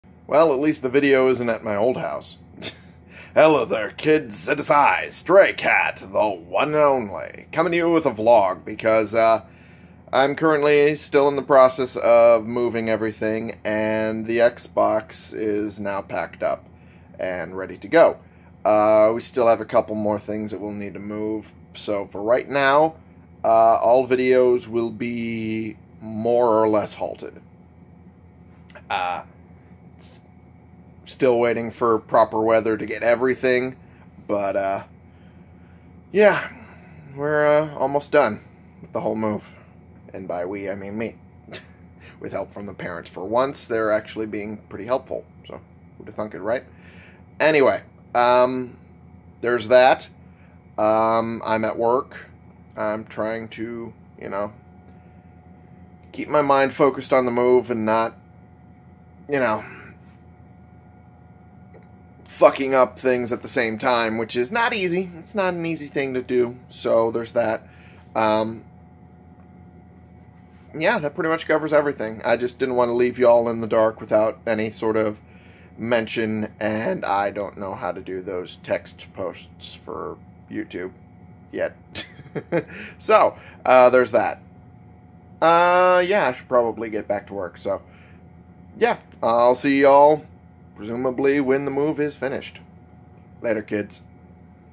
Just A Quick Update.
The move is taking about as long as I thought, so this is the hiatus for now! Just figured I would give you all the heads-up!